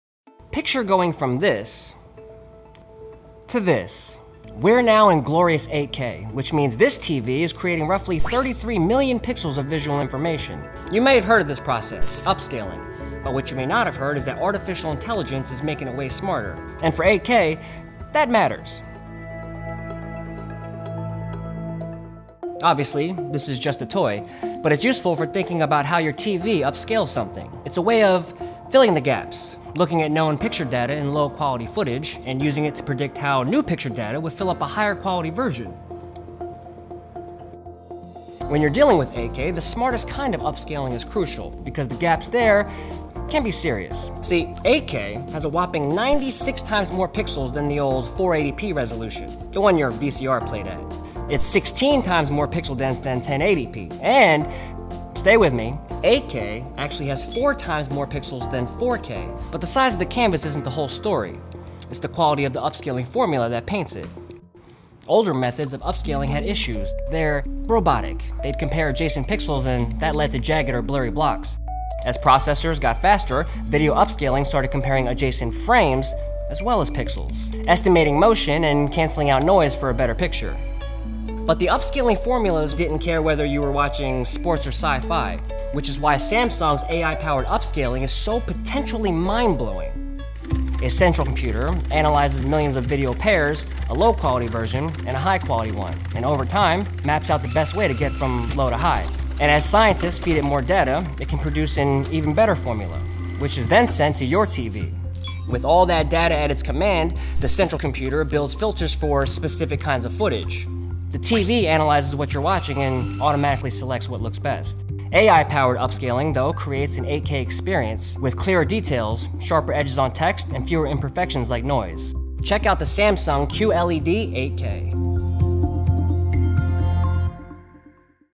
Using an AI-created formula to make upscaling smarter. [Advertiser content from Samsung]
We’ve all heard the phrase “bigger is better.” But size isn’t what makes Samsung’s new QLED 8K TV stand out in a crowded marketplace. The differentiator is thanks to a process called upscaling, and Samsung’s QLED 8K TV’s use of artificial intelligence to enhance the way upscaling is performed. Not only does its AI-powered upscaling* produce 8K image resolution, it upholds that resolution on a dynamic 75” screen.

Upscaling is especially critical when it comes to experiencing 8K because native 8K video content isn’t readily available yet. Samsung’s AI-powered upscaling* is able to transform HD/4K video content to 8K quality. The quantum processor uses machine learning and artificial intelligence to produce detailed resolution while also creating an immersive and realistic experience.

So how does it work? A computer analyzes millions of video pairs — a low quality version and a high quality one — and over time maps out the best way to get from low to high. As scientists feed it more data, it can produce an even better formula, which is then sent to your TV.